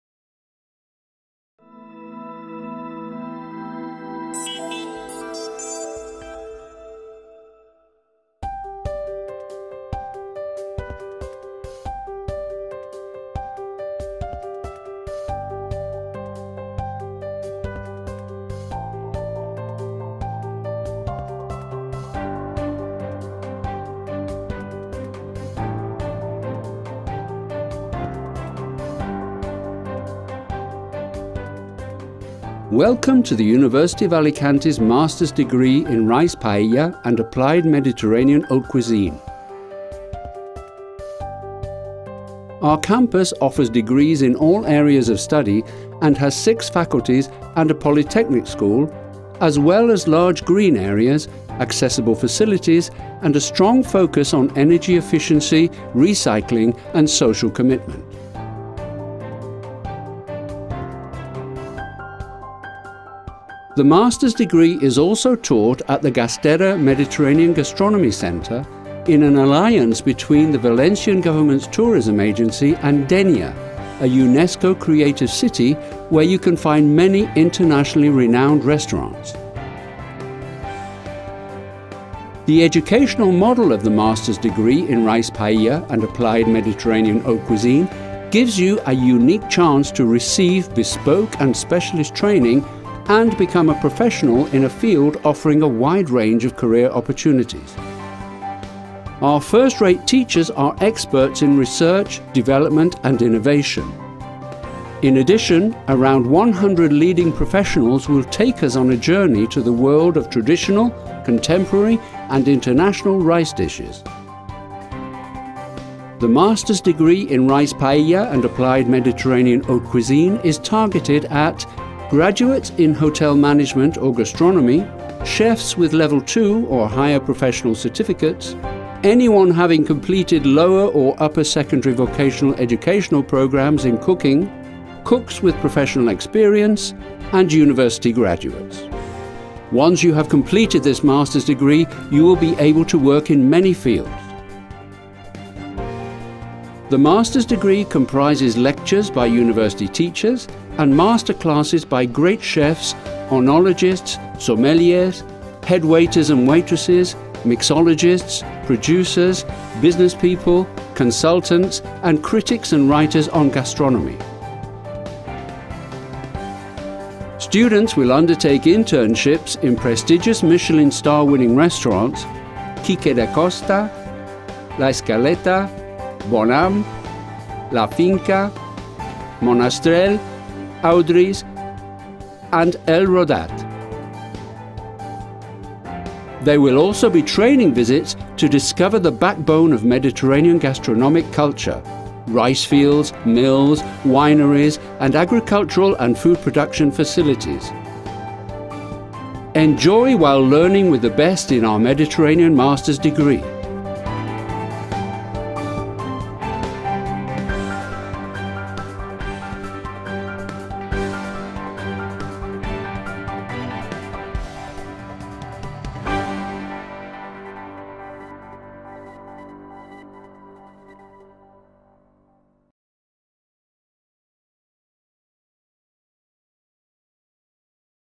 Master in Rice and Mediterranean Haute Cuisine presentation
The Master puts the students in a unique training in a professionally demanded field. Students can apply the new techniques of Haute cuisine to the cooking of rice, developing creative and sensory skills for the creation of contemporary rice and acquiring a broad base on the physical-chemical processes involved in the development of them. The students will specialize in Mediterranean Haute cuisine and in the elaboration of rice dishes knowing their implantation in the Spanish gastronomy. You can acquire fundamental knowledge about traditional rice cuisine. You will know the main products that are involved in the preparation of rice; raw materials, production areas, processing, peculiarities, cooking techniques, etc. The classes will be taught by high level teachers. More than 50 professional chefs of reference, recognized with all kinds of distinctions and specialized in Haute cuisine and in the preparation of paellas and rice dishes that will be taught: Masterclass, master classes, practical sessions and training visits.